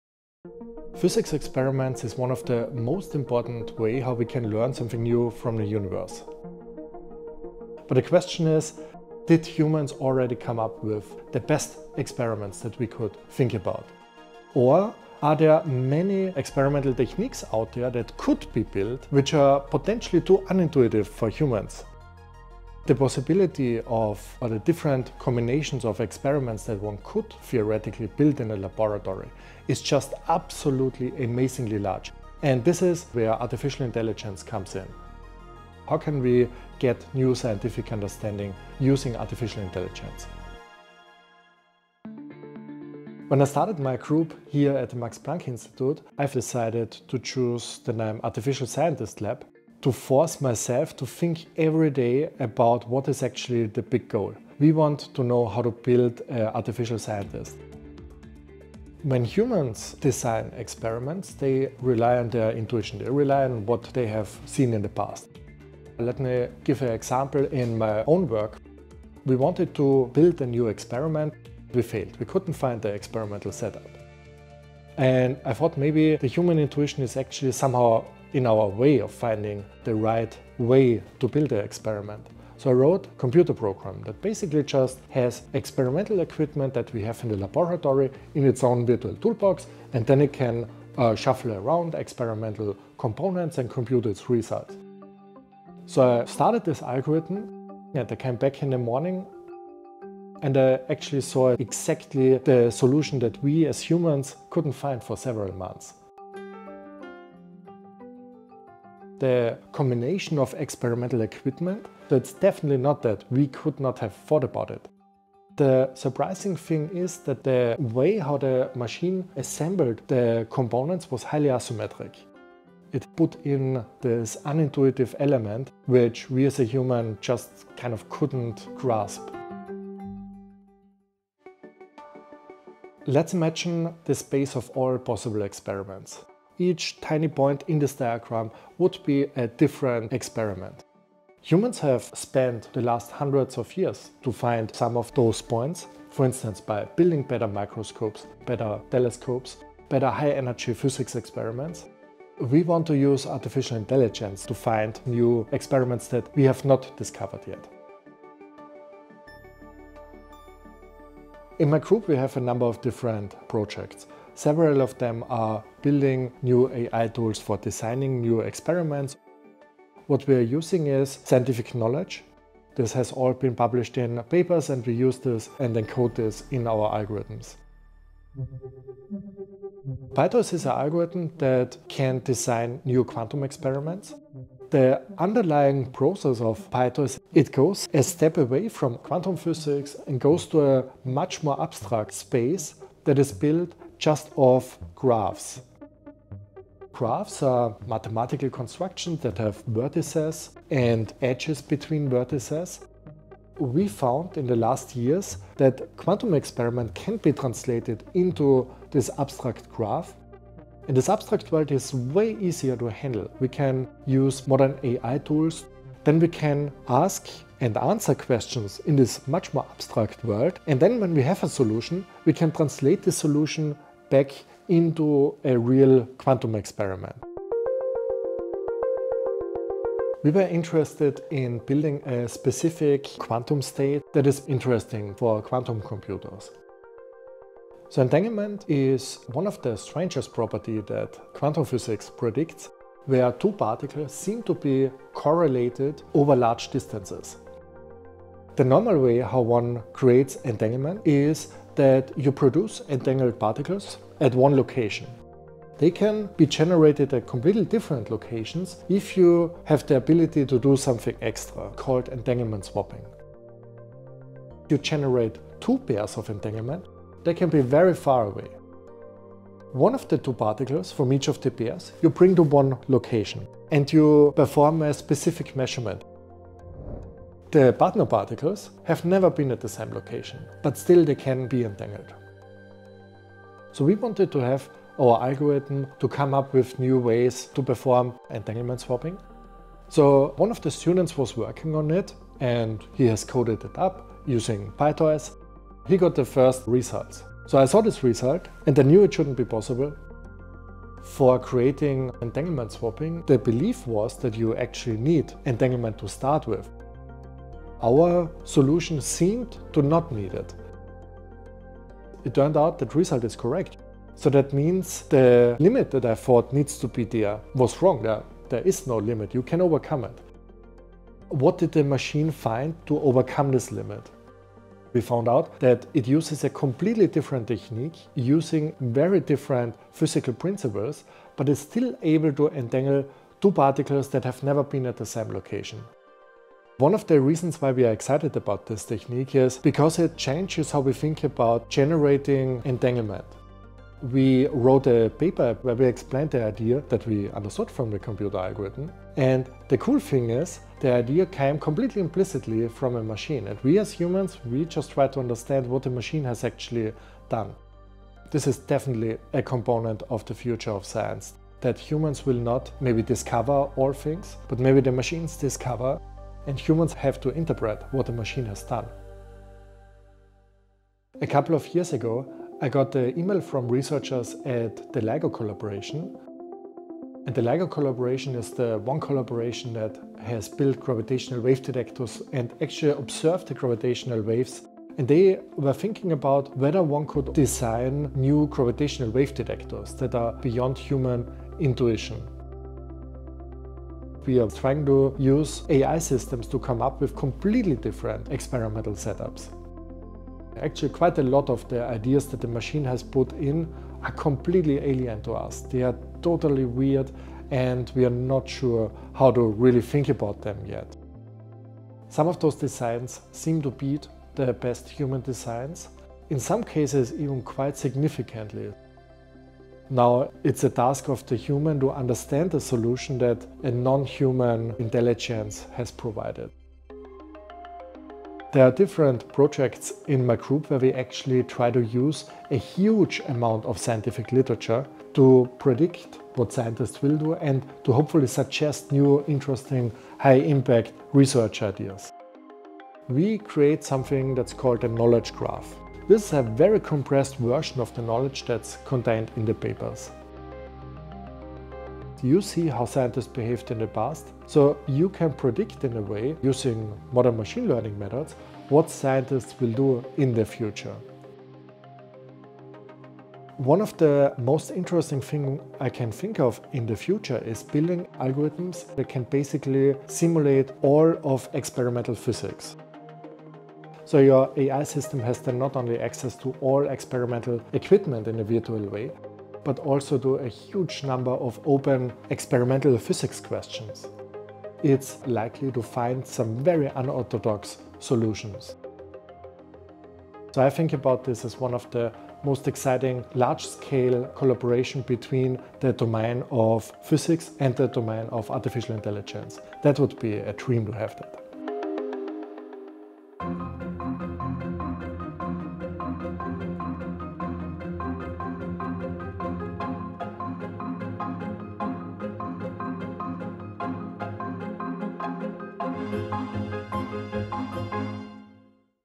The AI Breakthrough That Finds 'Impossible' Experiments
Physicist Mario Krenn uses artificial intelligence to inspire and accelerate scientific progress. He runs the Artificial Scientist Lab at the Max Planck Institute for the Science of Light, where he develops machine-learning algorithms that discover new experimental techniques at the frontiers of physics and microscopy. He also develops algorithms that predict and suggest personalized research questions and ideas.

00:00 - The Artificial Scientist Lab
00:59 - The limits of human intuition
02:12 - Building algorithms to design experiments
03:37 - Algorithm makes a discovery about entanglement
06:11 - Collaboration with LIGO (gravitational wave detectors)
07:07 - Using AI to generate research ideas
- FOLLOW us Twitter:  / quantamagazine

Quanta Magazine is an editorially independent publication supported by the Simons Foundation